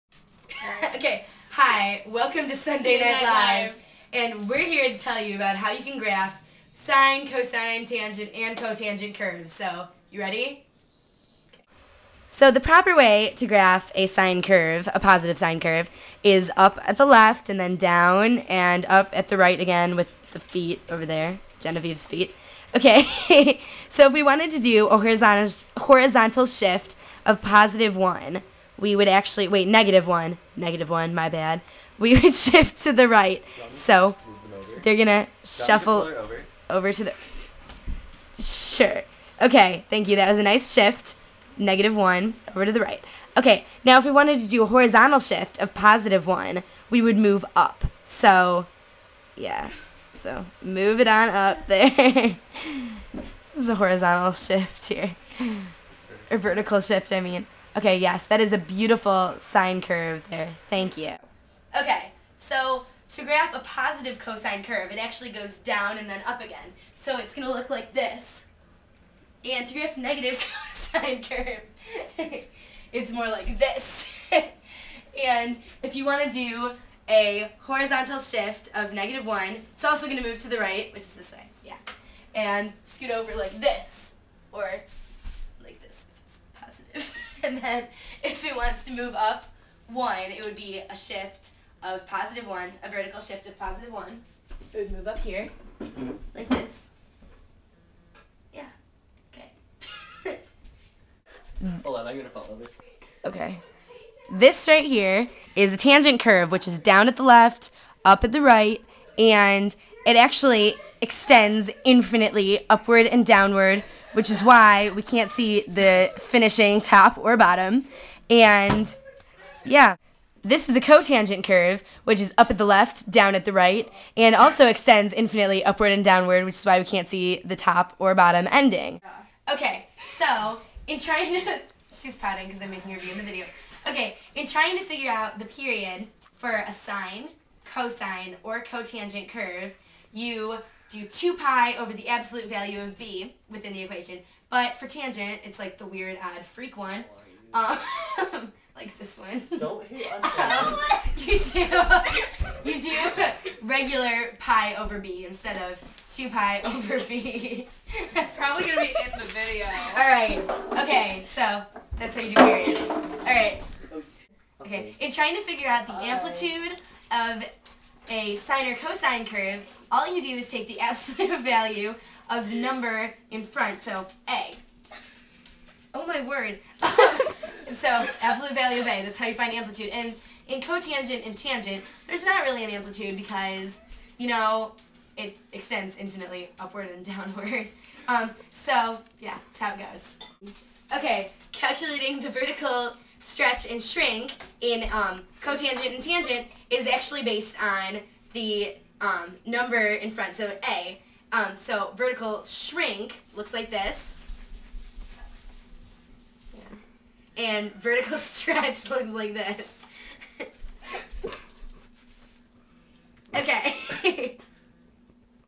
Sin, Cos, Tan, & Cot Graphing, the Fun Way
People form great graphs of Trig functions.